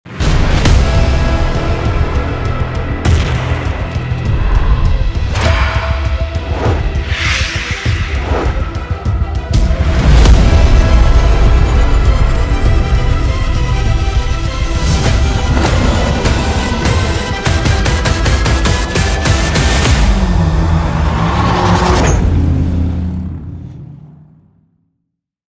[4K] Epic sport car B-roll recorded with iPhone
Hey, please enjoy my latest b-roll.
Gear: iPhone 11 + DJI pocket mobile 3 gimbal + Mavic Mini.